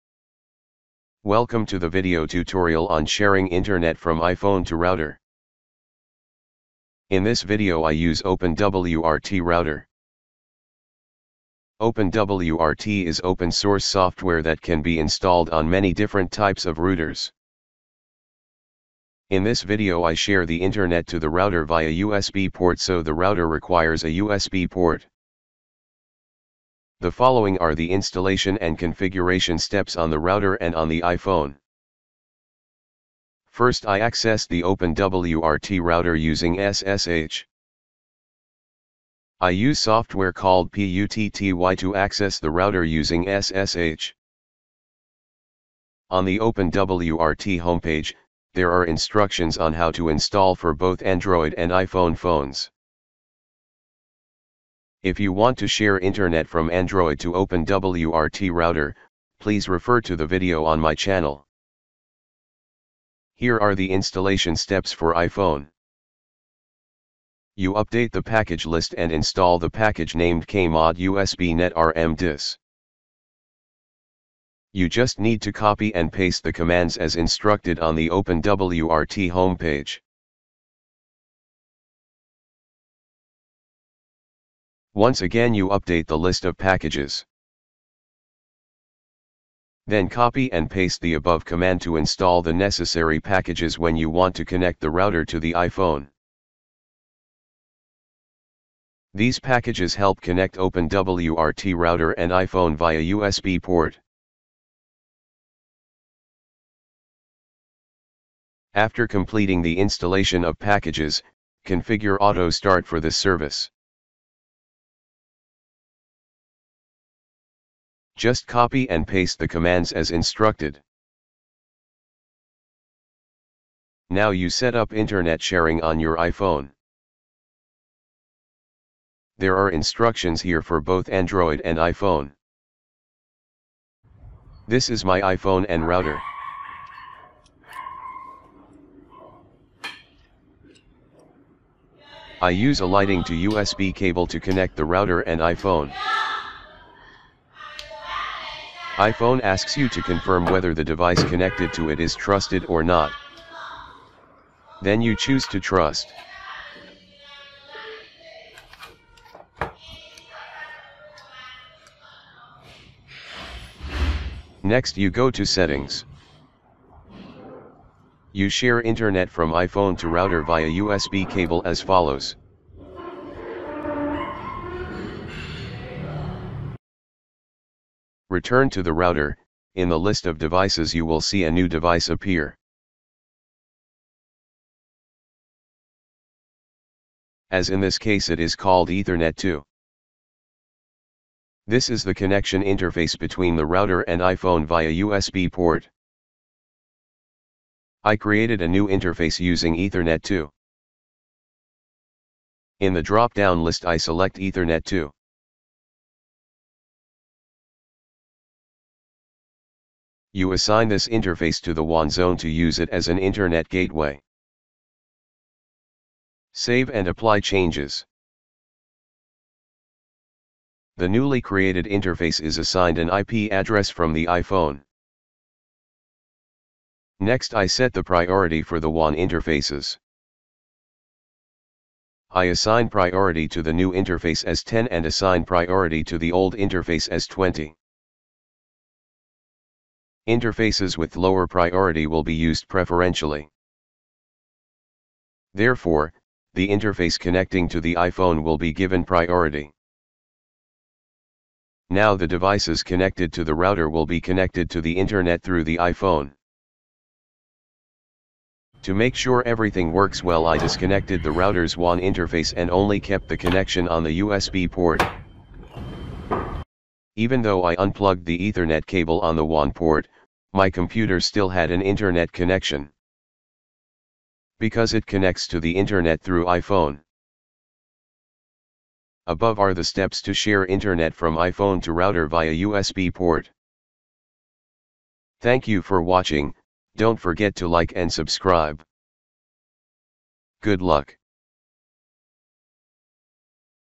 How To Share Internet from iPhone with OpenWRT
In this step-by-step video, we will show you how to share your iPhone's internet connection with a router using a USB cable. This method is incredibly useful if you have a limited data plan on your iPhone but need to connect multiple devices to the internet through a router.

By the end of this video, you'll have a reliable and convenient way to share your iPhone's internet connection with all your devices.